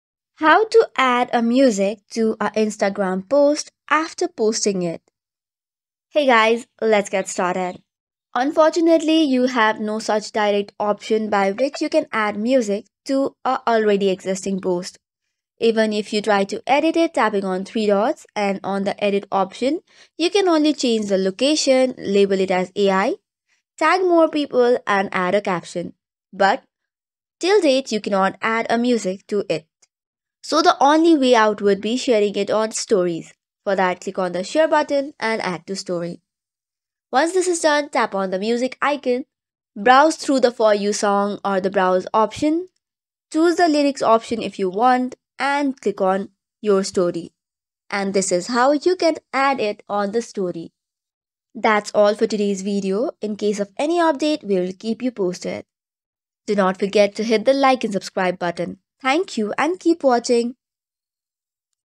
How To Add Song To Instagram Post After Posting It
Forgot to add a song to your Instagram post before posting it? No worries! Learn how to add a song to your Instagram post after it's been published with this easy guide. Enhance your post with music and keep your followers engaged.

In this tutorial:
0:00 - Introduction
0:05 - How to Add Song To Instagram Post After Posting It

Discover the straightforward process of adding a song to your Instagram post even after it's been published. This comprehensive tutorial ensures you can easily edit your post to include music, allowing you to enhance your content and keep your followers entertained.

Easily add music to your Instagram posts after posting them by following this guide!